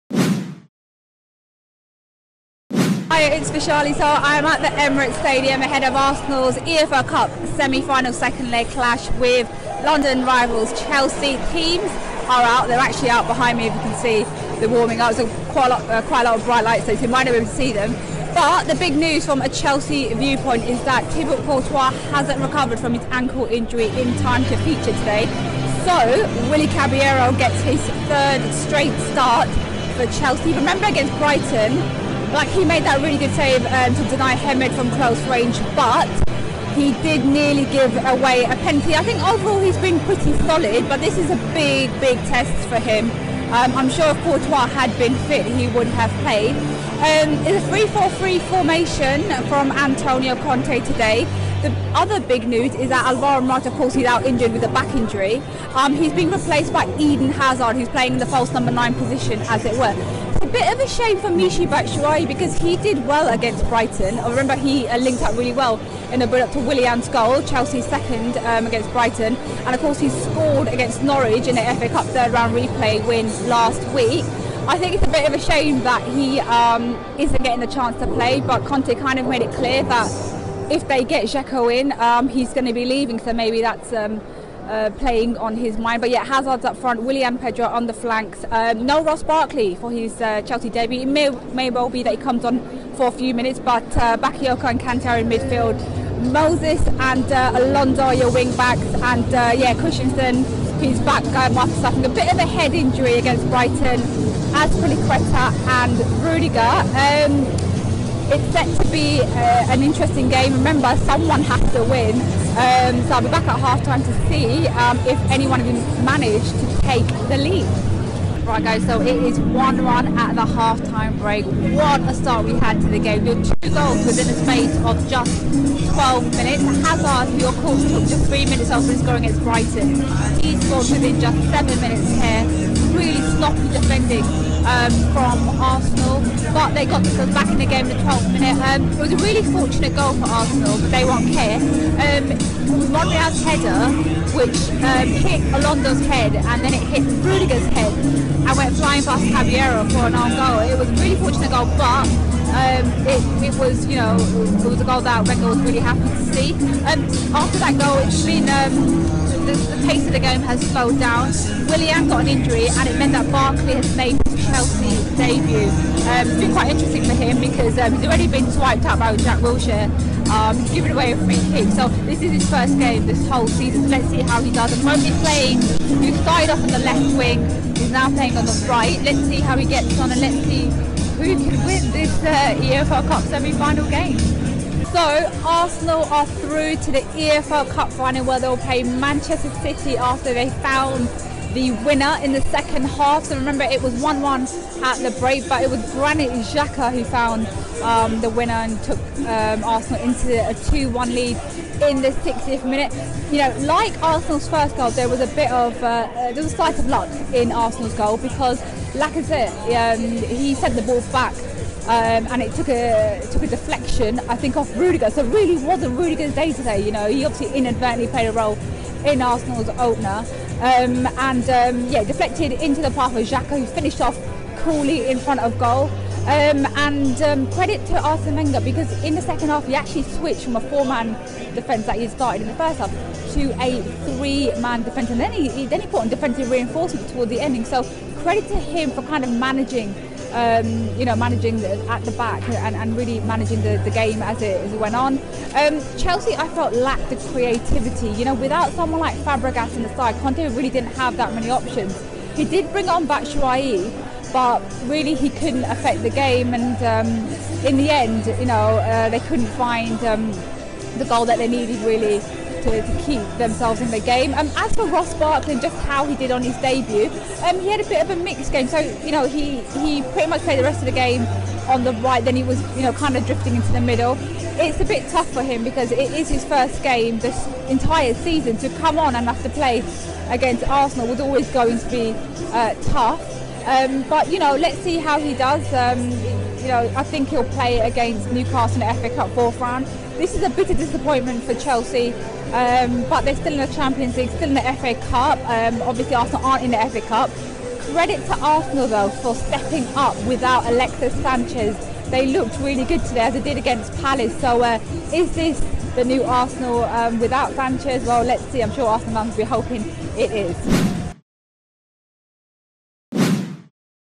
Arsenal 2-1 Chelsea EFL Cup matchday vlog | Ross Barkley and Arsene Wenger tactical analysis
Apologies for the late uploading of this, but do check out my matchday vlog from the Emirates Stadium focusing on how Ross Barkley did on his debut.

Video editing by Ravit Anand

Follow him!